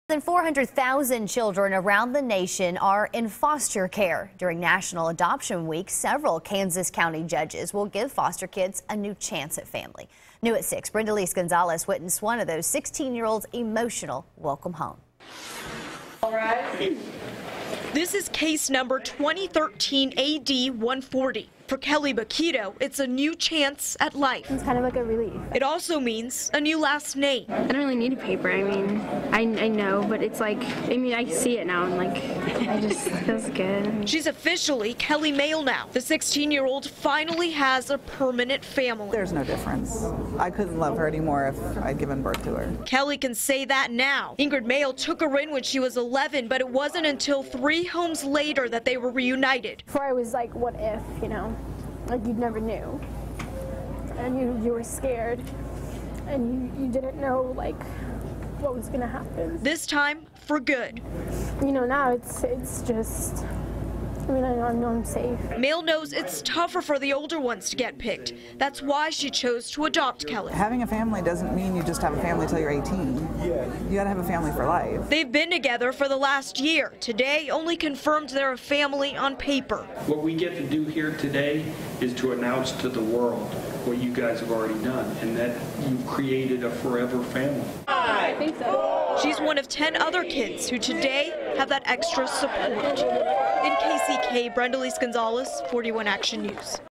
16-year-old welcomed home as part of National Adoption Week
During national Adoption Week, several Kansas county judges will be giving foster kids a new chance at family